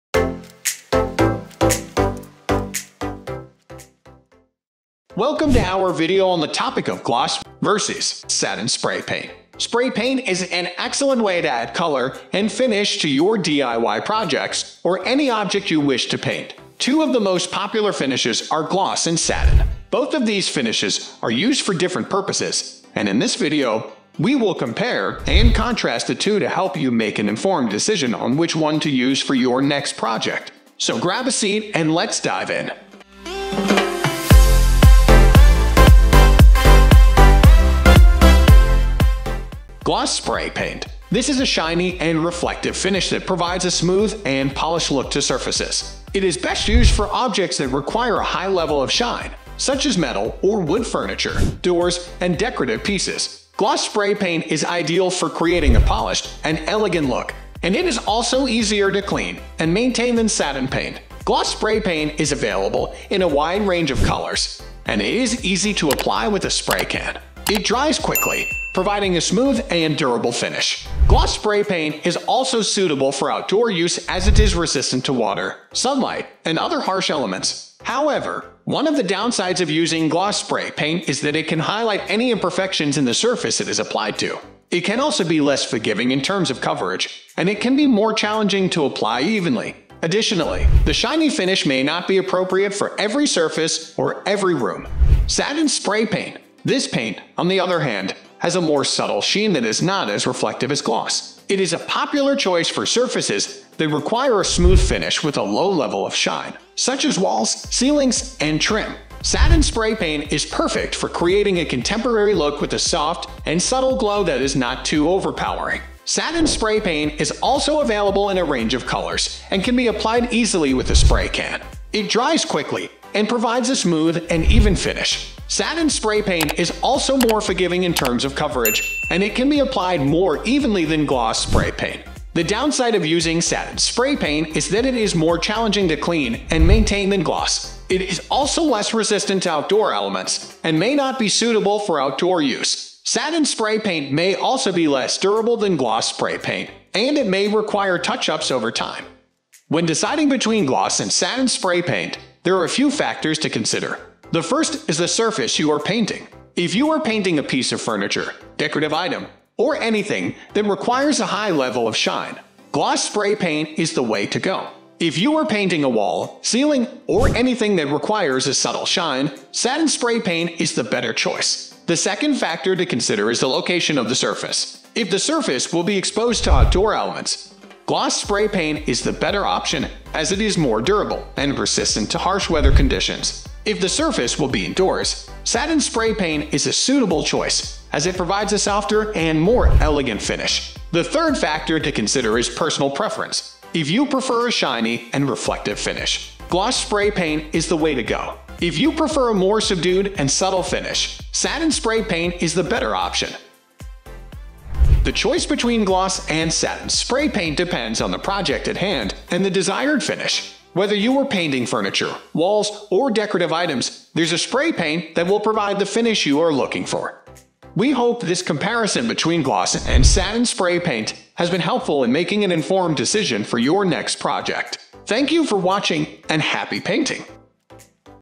Gloss vs Satin Spray Paint: Which Finish is Right for You?
Are you in search of the perfect spray paint finish for your next DIY project? Look no further! In this comprehensive video guide, we dive deep into the differences between gloss and satin spray paint finishes, helping you make an informed decision and achieve the desired look for your project.